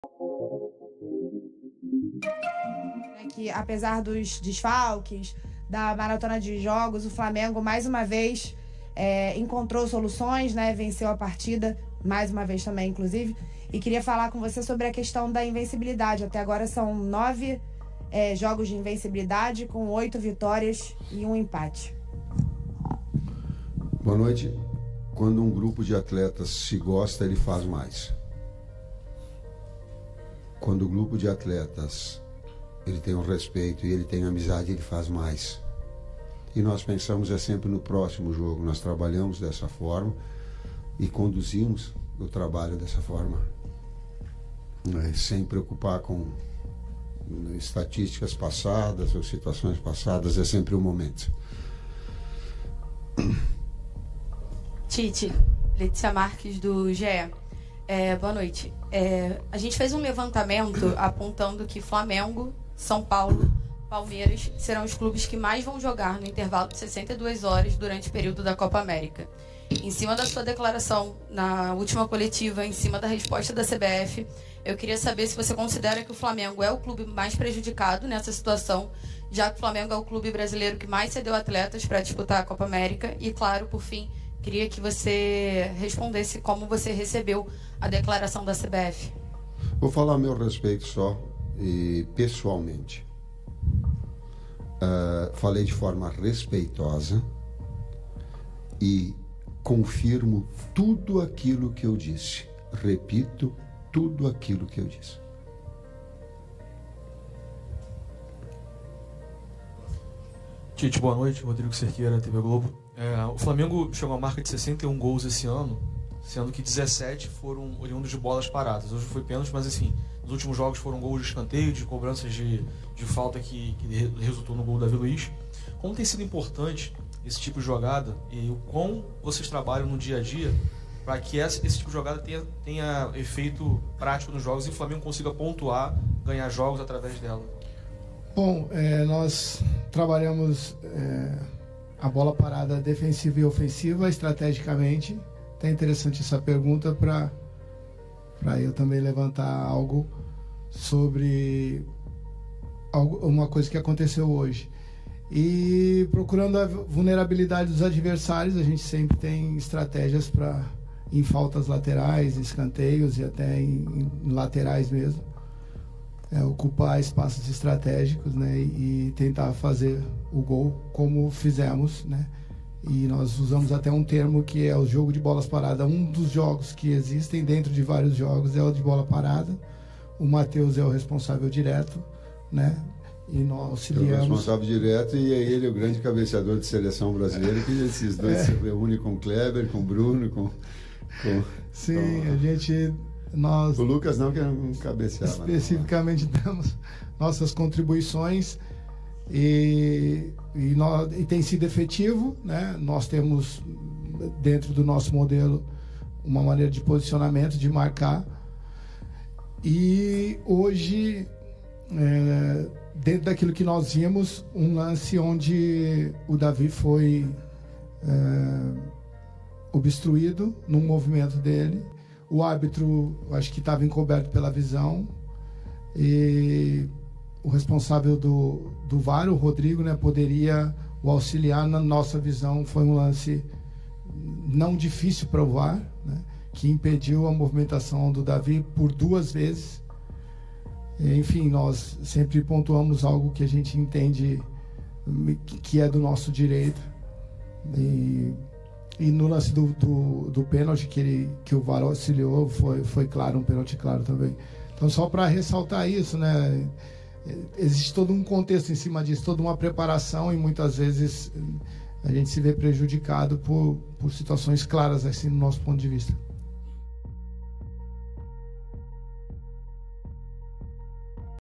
TITE FALA SOBRE O JOGO APÓS VITÓRIA | FLUMINENSE 0X1 FLAMENGO | BRASILEIRÃO 2024
e e não perca mais tempo, comece a ter uma renda extra através do YouTube hoje mesmo!"

Nesse vídeo, veja notícias do Flamengo, o técnico Tite em entrevista coletiva fala sobre o jogo.